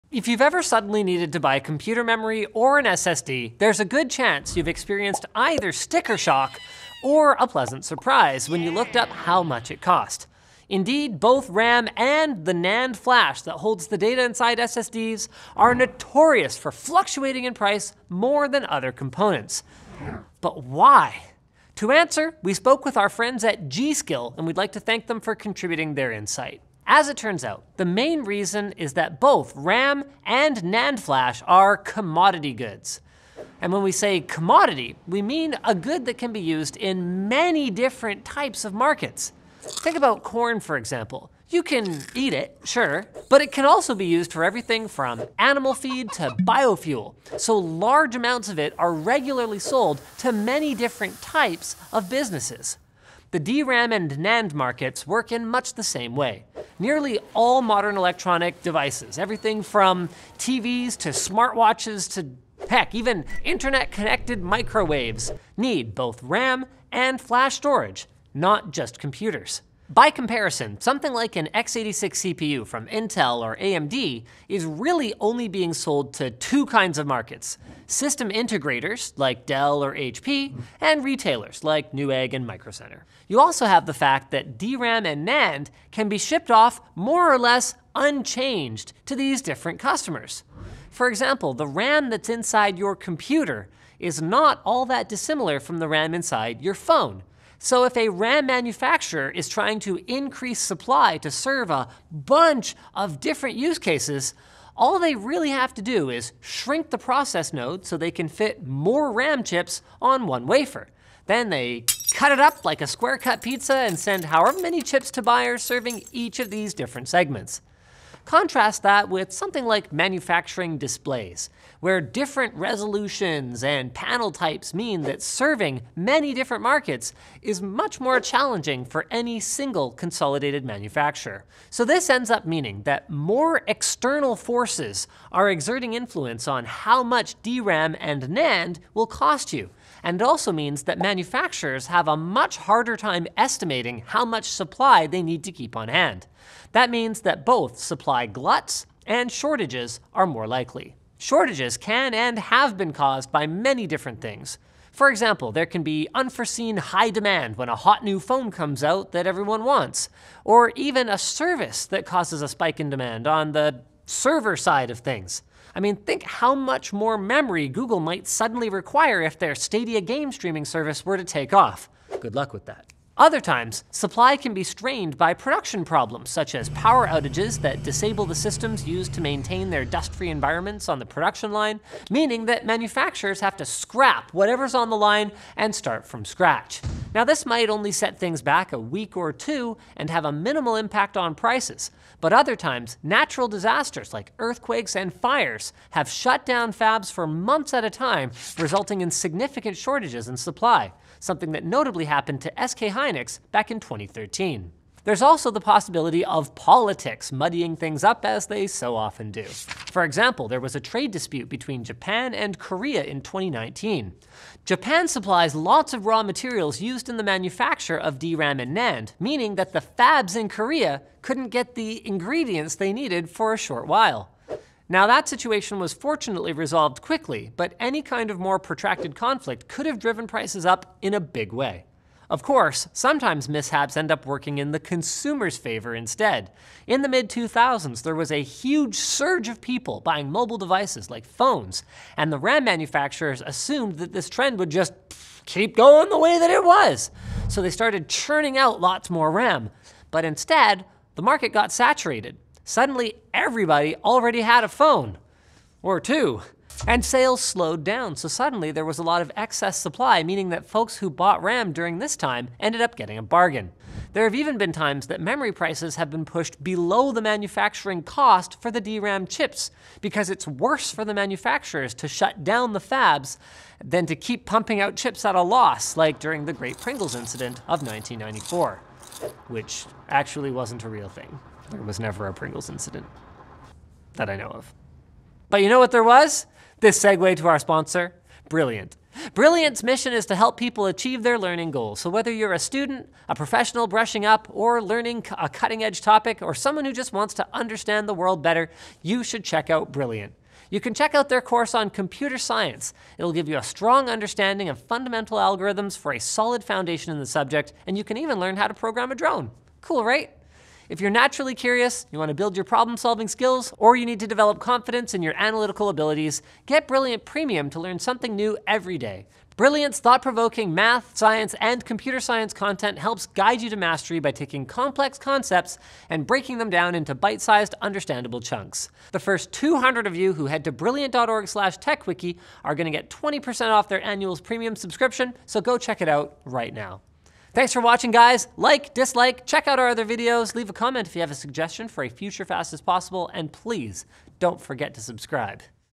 Why Do RAM Prices CHANGE So Much?
RAM and flash memory prices seem to go up and down a lot - but why?

Buy RAM
On Amazon (Paid Link)
On Newegg (Paid Link)
On BHPhoto (Paid Link)